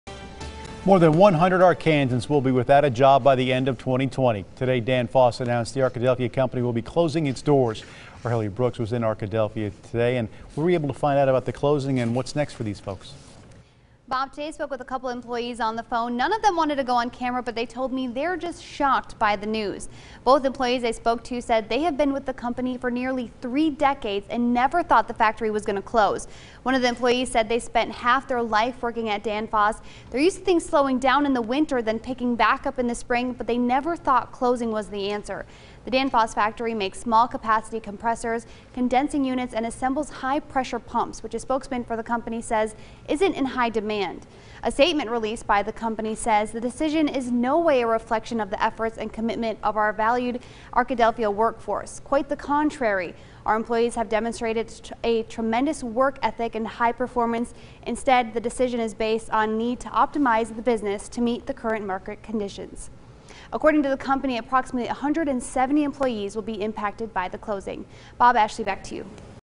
Danfoss announces closure to Arkadelphia factory by end of 2020, 170 employees to be furloughed
Danfoss announced Tuesday it plans to close its Arkadelphia compressor factory by the end of 2020.